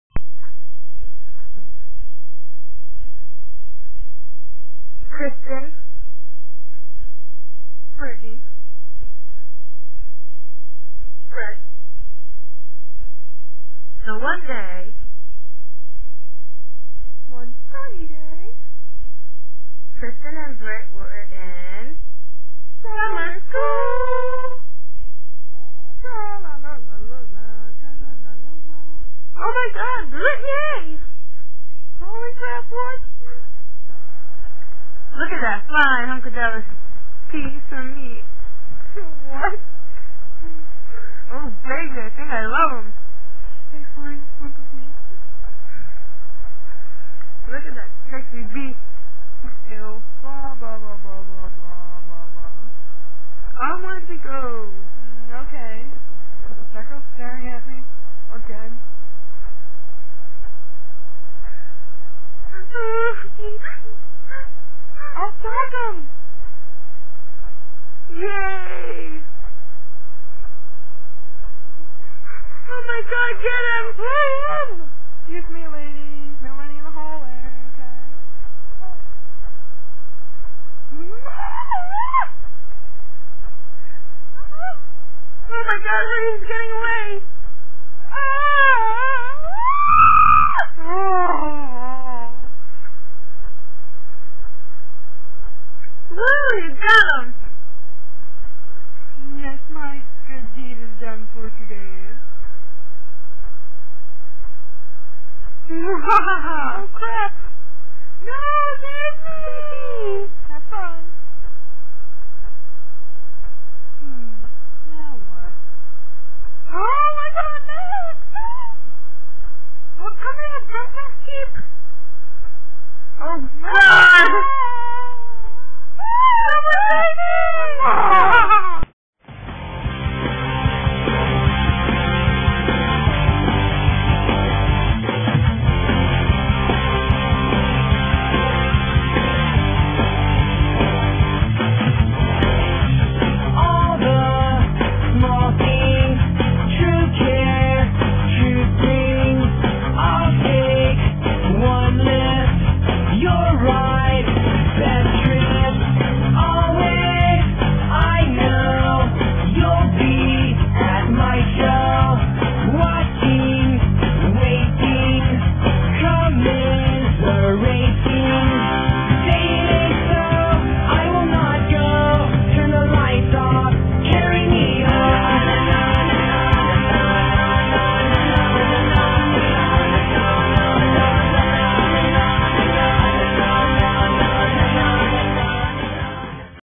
bleh
Me and Kristin got bored.
I'm the voice of me [[Britt]], Brett and Bob Ross. Kristin is herself and Matt...